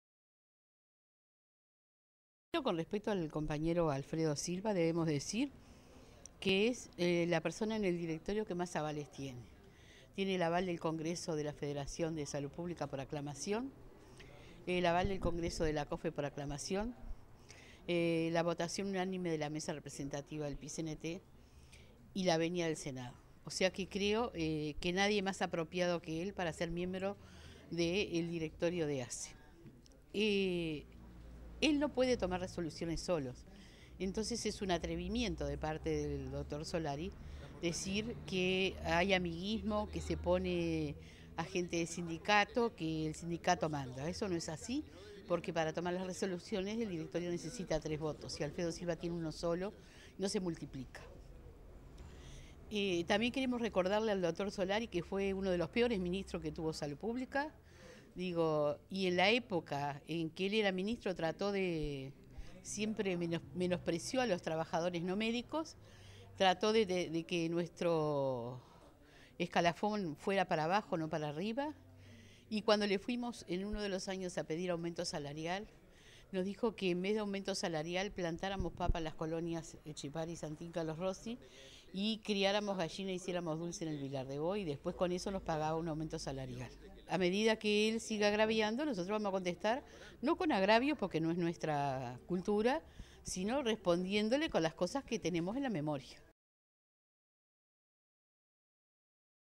VTV NOTICIAS: FAGIAN
Desde la Federación de Funcionarios de Salud Pública se repudió los dichos del senador Alfredo Solari.
En tal sentido la gremial dijo que sus acusaciones fueron una falta de respeto, y que Solari, fue "el peor Ministro que tuvo Salud Pública".